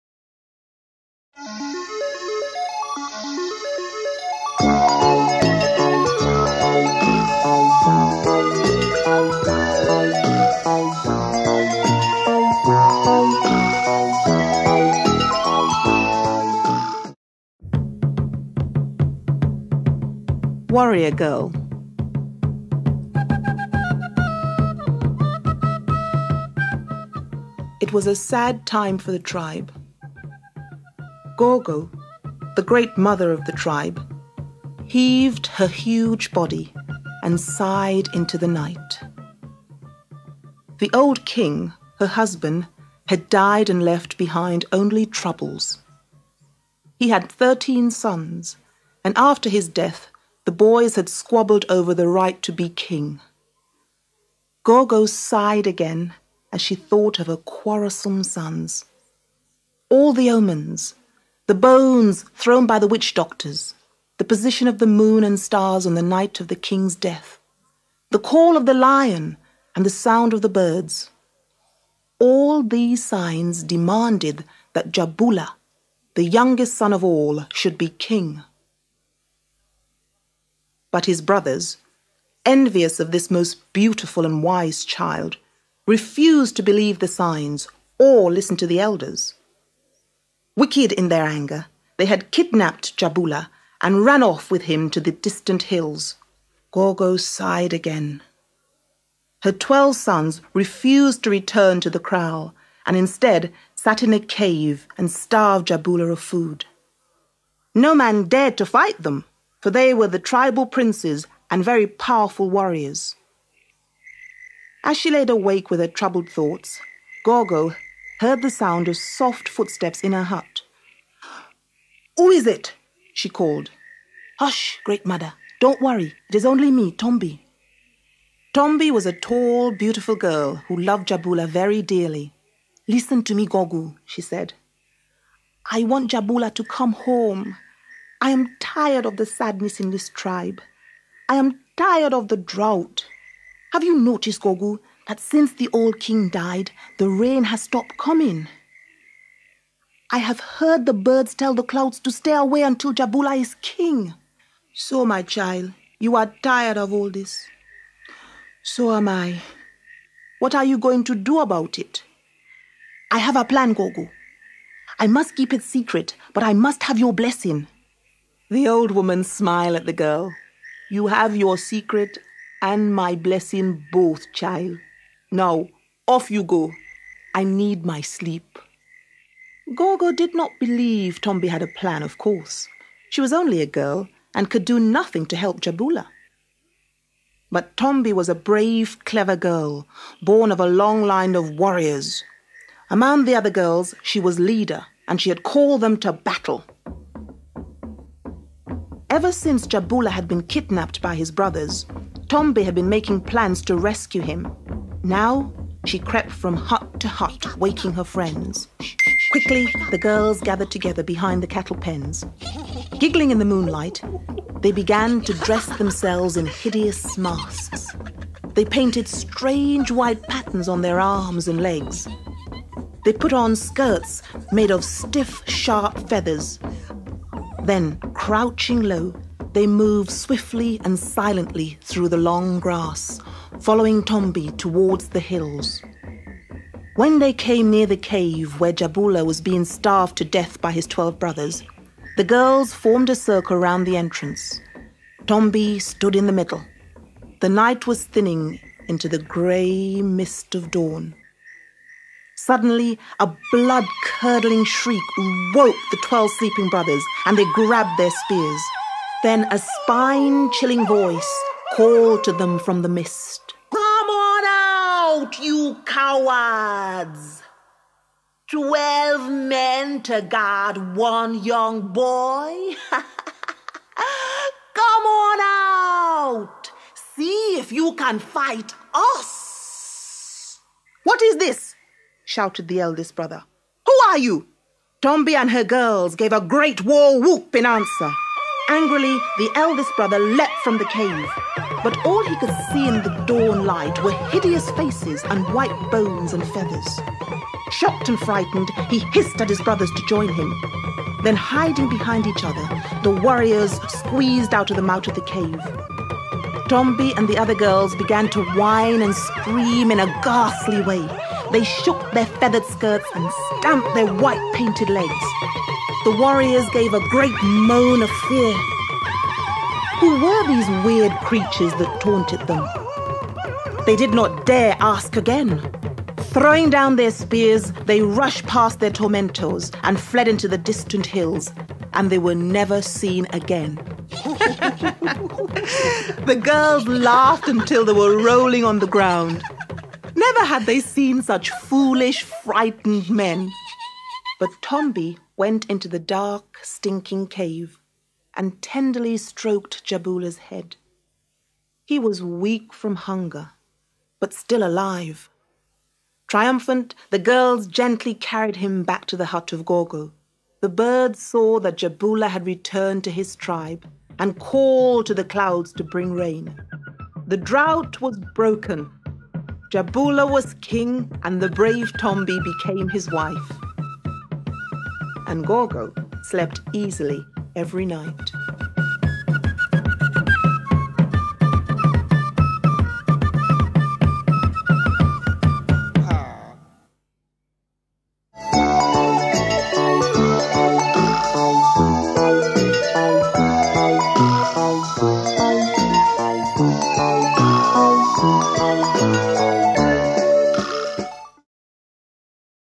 Warrior Girl
0:16 - Tombi Fools the warriors and ends a sad chapter in the life of her tribe.  This original story, set in the African veldt, is written and illustrated by Tessa paul
6:18 - Written and Illustrated by Tessa Paul, Read by Floella Benjamin